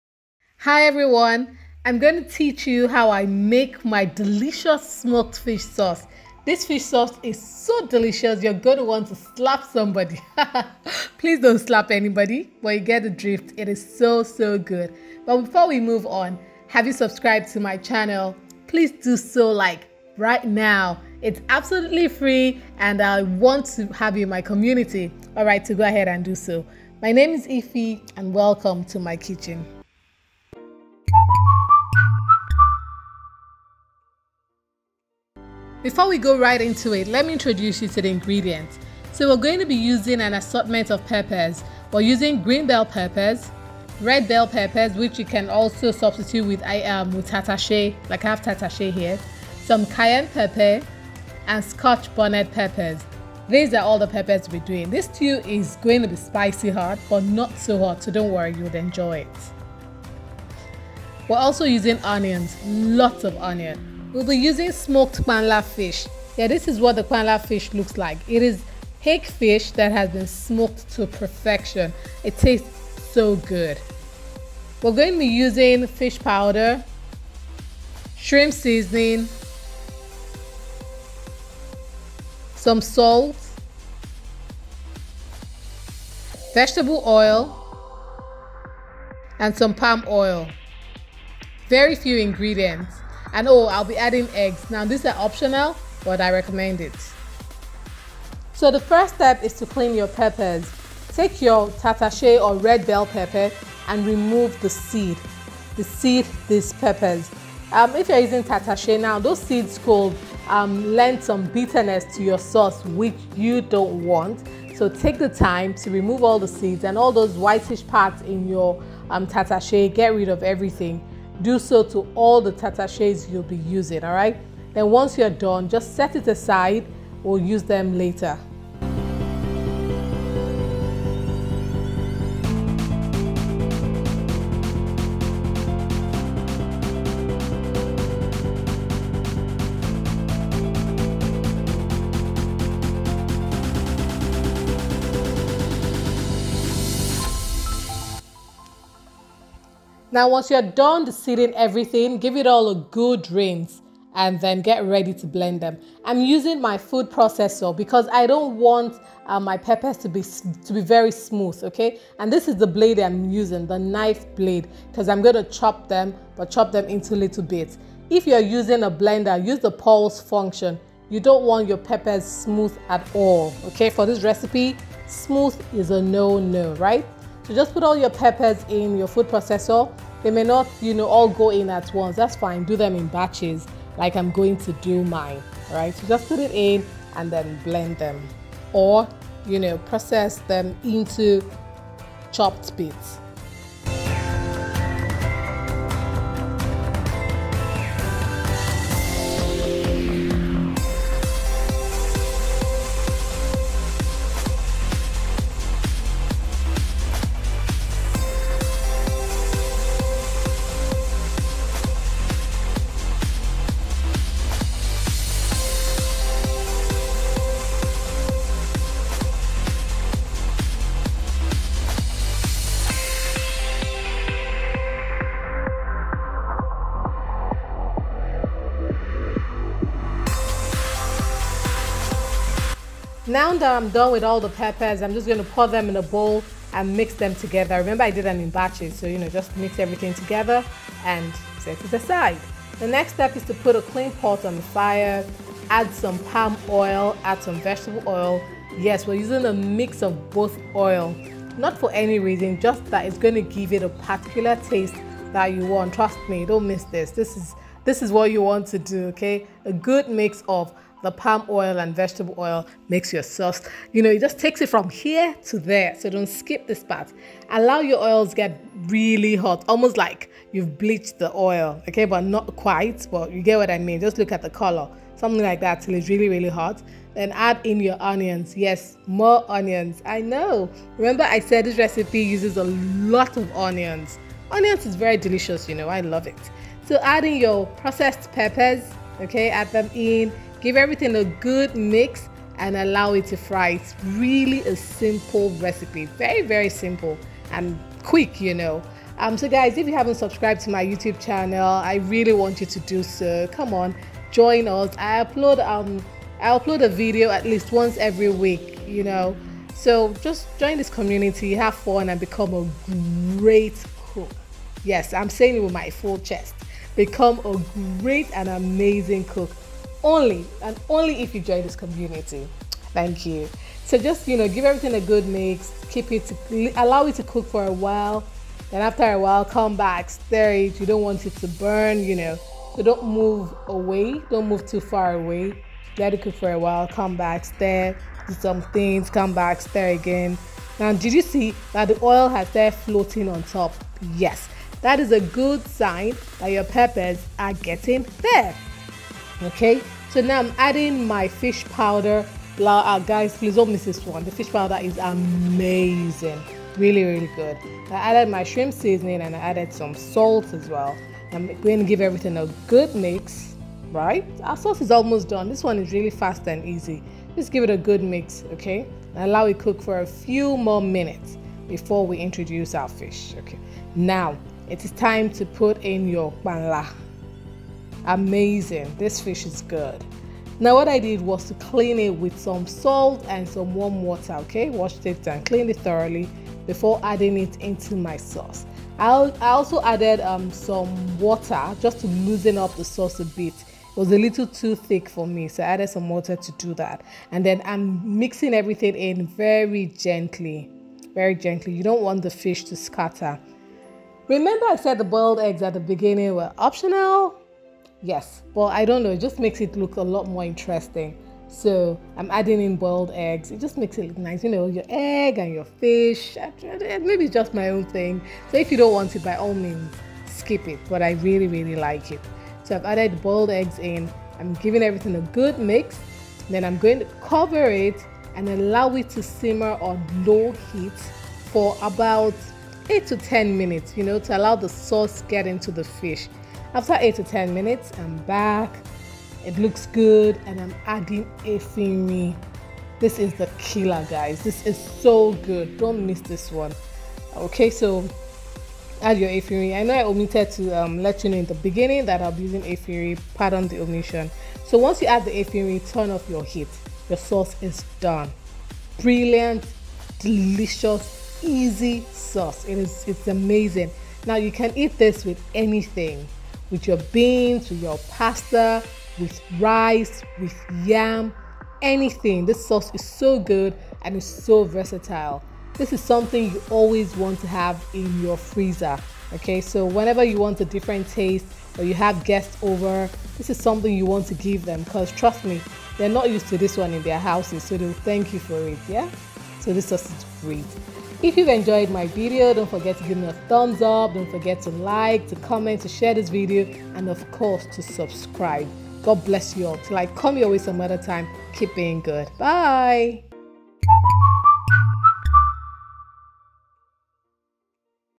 THE BEST NIGERIAN SMOKED PANLA FISH SAUCE/ PERFECT SPICY FISH STEW/PEPPERED FISH
This perfect smoked fish  sauce is nothing like you have ever had. It is spicy, delicious and pleasing to the eyes: just how Nigerians love their fish sauce. It is the best and perfect Nigerian fish sauce/stew out here. You can eat it with Rice, beans, yam, pasta etc.

Ingredients for the best Nigerian smoked fish stew/sauce
2 green bell peppers
5 large red tatashe
10 Cayenne pepper
10 scotch bonnets
4 onions
1/4 cup palm oil
1/2 cup vegetable oil
Shrimp seasoning powder
2 tablespoons fish powder
5 smoked panla fish
3 boiled eggs
5-6 Effirin/Scent leaf/Nchawu

Thank you and I love you guys.